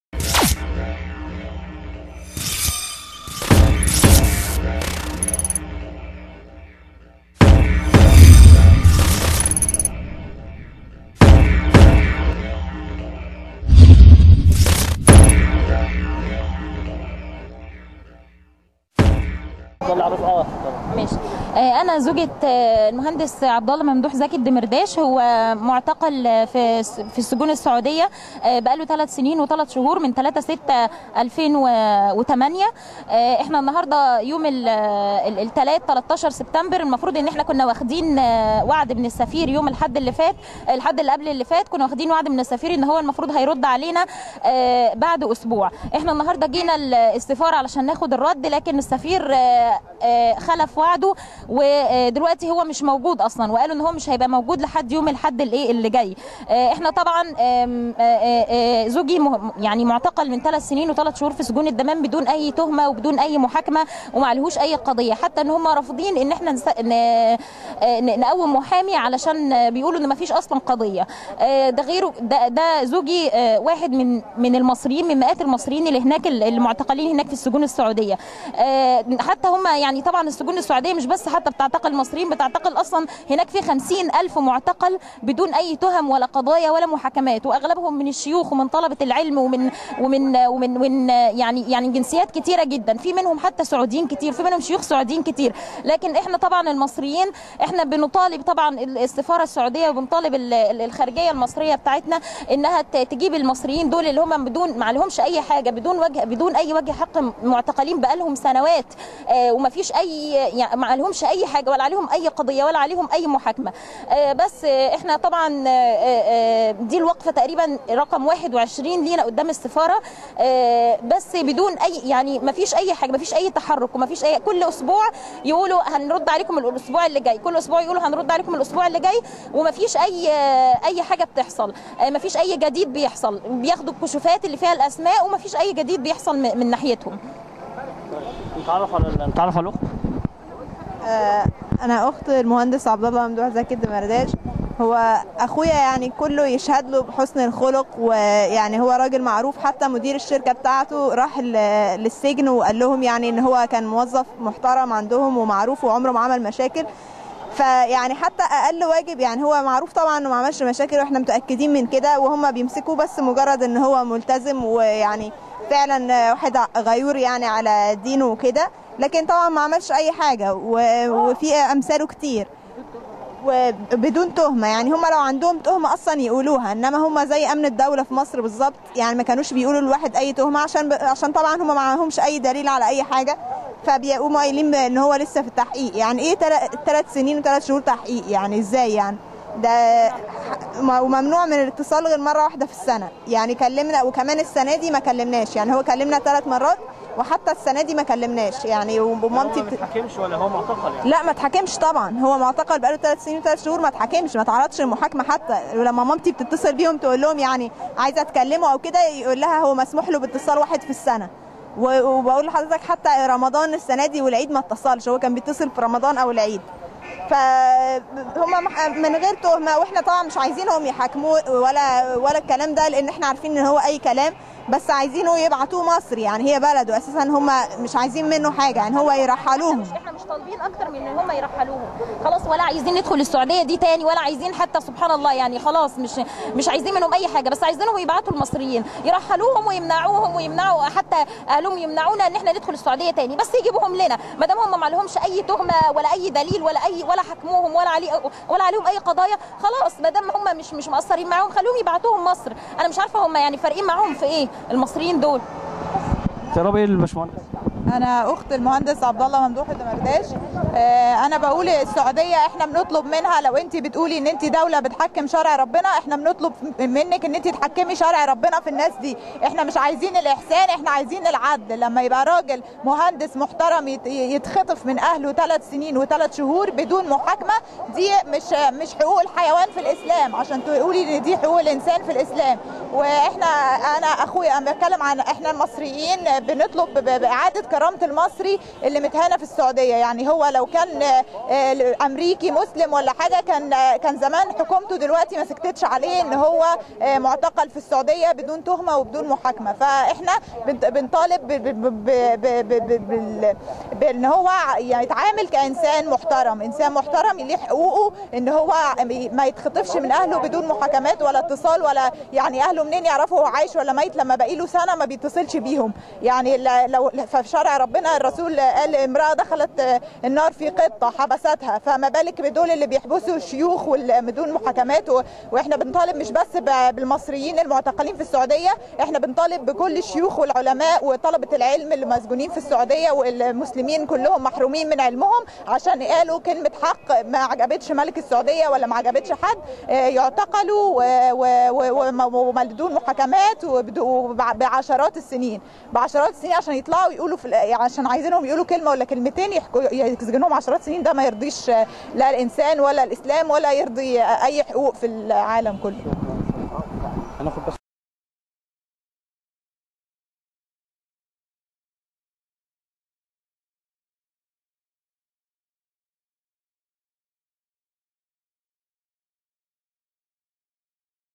السفير السعودى يتناسى المصريين المعتقلين بالسعودية
السفير السعودى يتناسى المصريين المعتقلين بالسعودية منذ سنوات , والمتظاهرين أهالى المعتقلين يستنكرون موقف السلطات المصرية من عدم إتخاذ اى إجراءات بشأنهم و يتهمون السفير السعودى بخلف وعوده بمقابلتهم